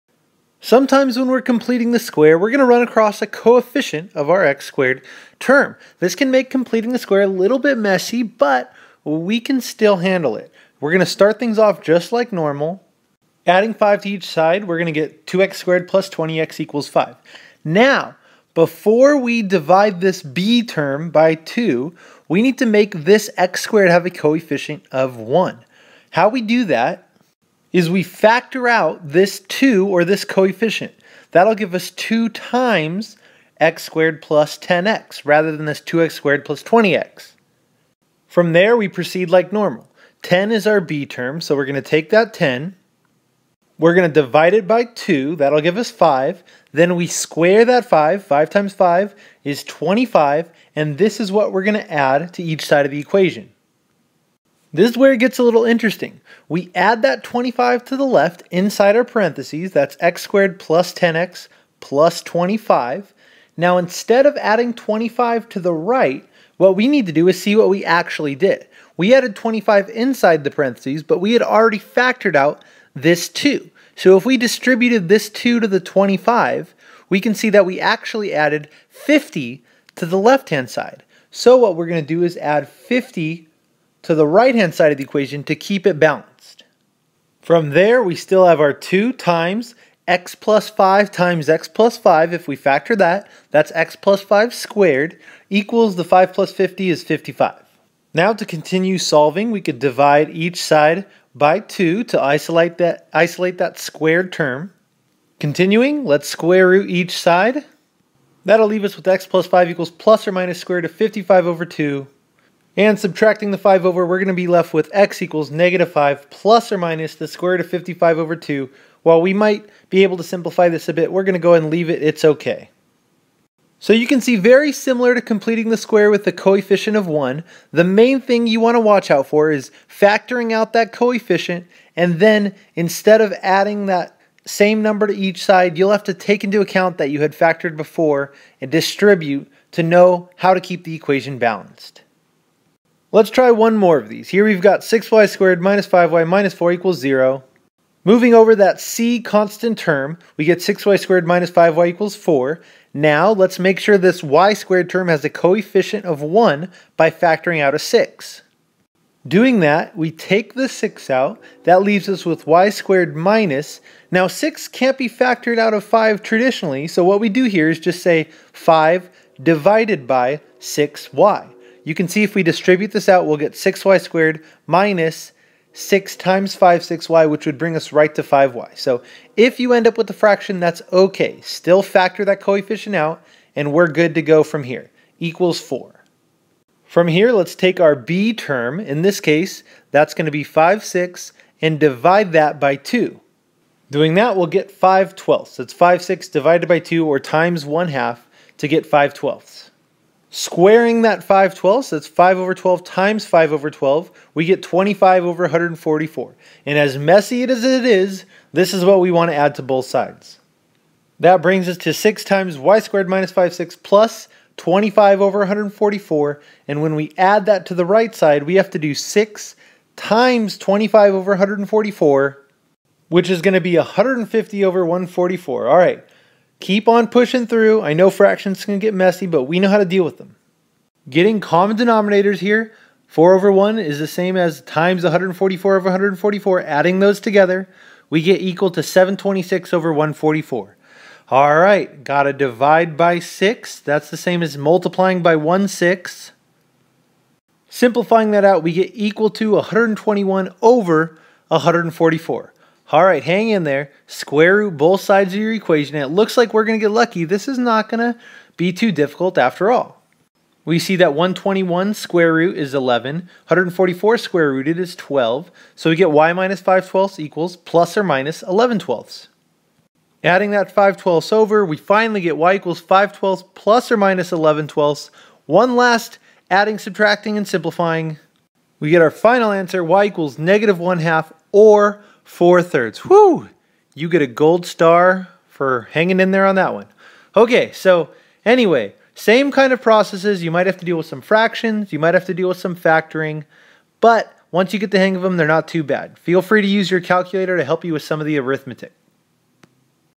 Completing the Square with a Coefficient
How to complete the square when there is a coefficient other than one.